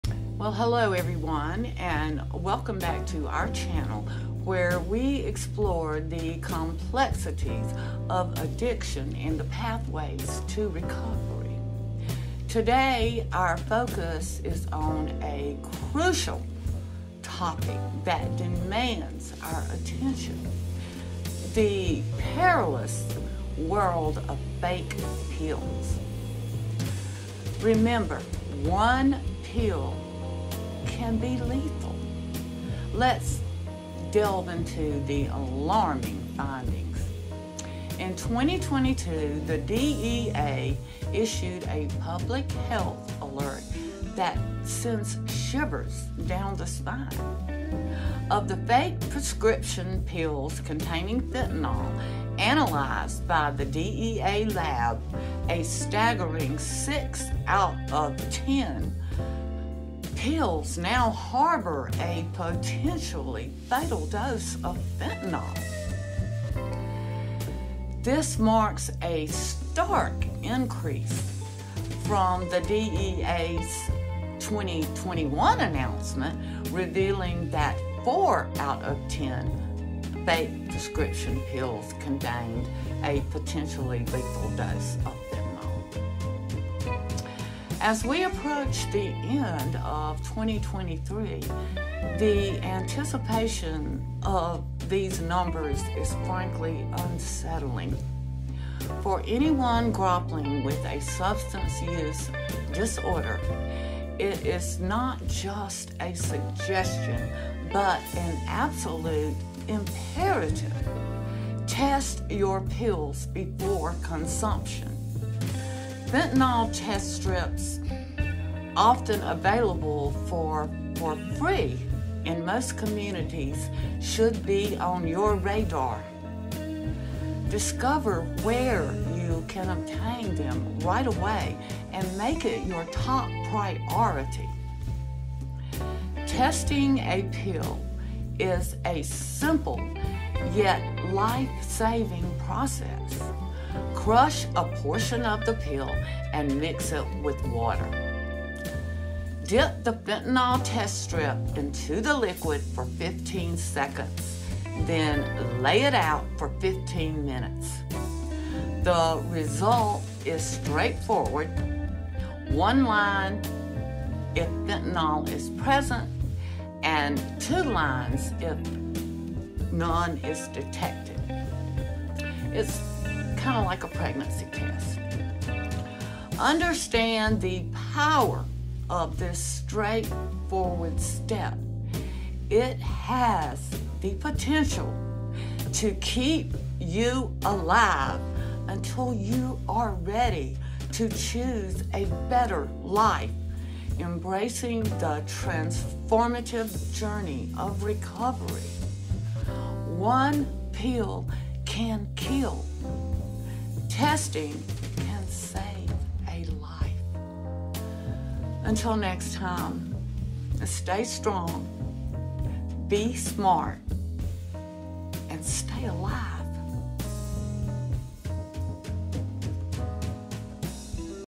DANGER!!!!
If you, or someone you love is struggling with a Substance Use Disorder, It is Imperative to test the pills before consumption. Testing pills is an easy straight forward process that could very well save your life until you are ready to commit to recovery.